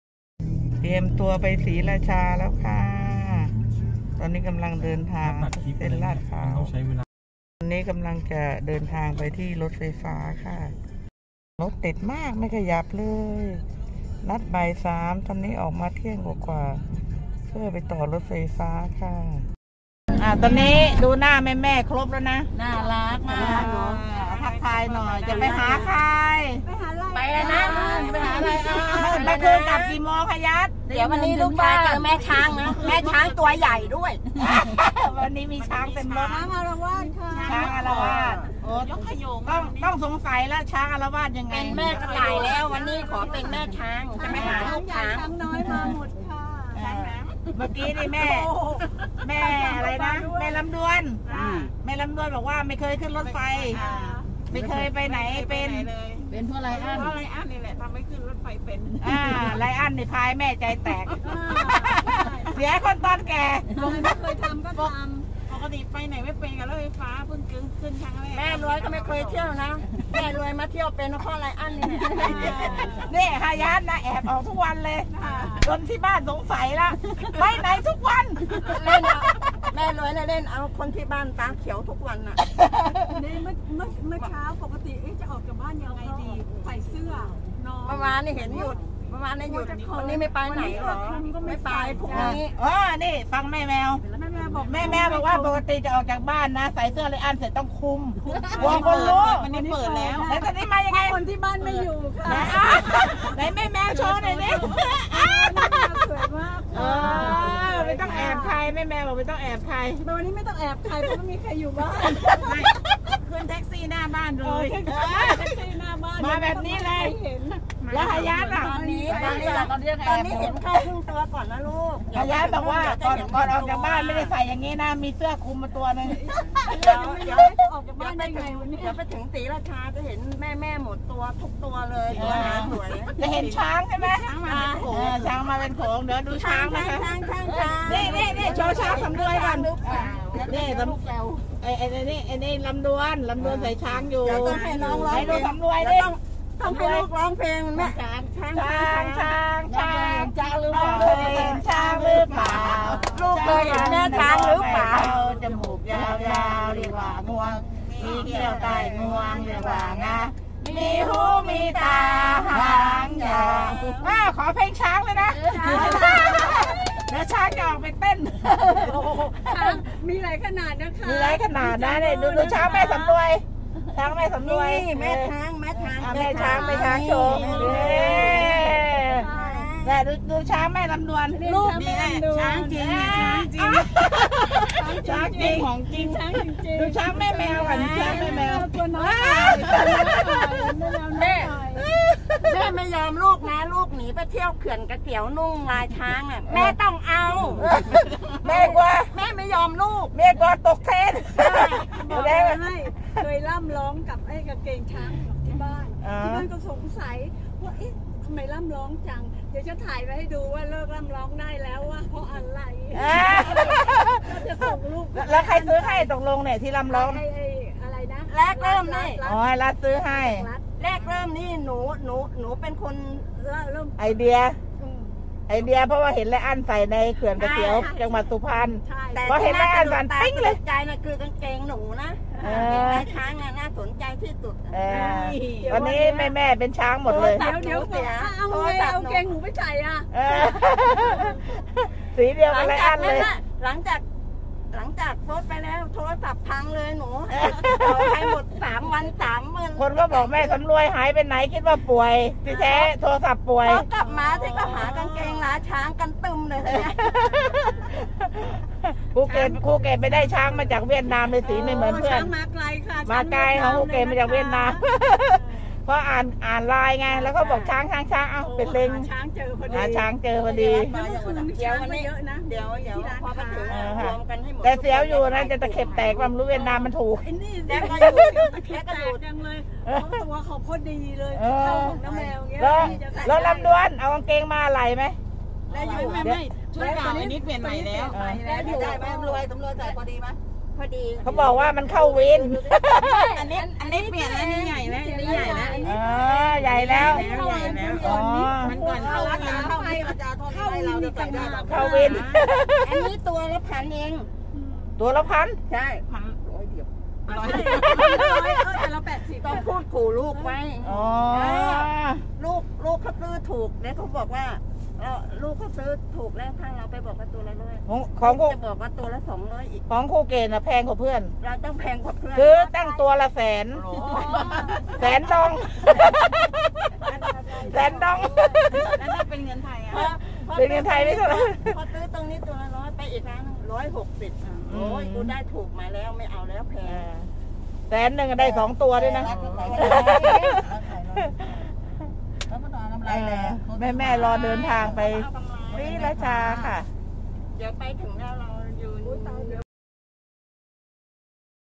รายงานสด จากชลบุรี ลุยต่อยังไม่ได้พัก ติดตามลูกชาย ชมคอนเสิร์ตคืนนี้ค่ะ
4 ธันวาคม 2565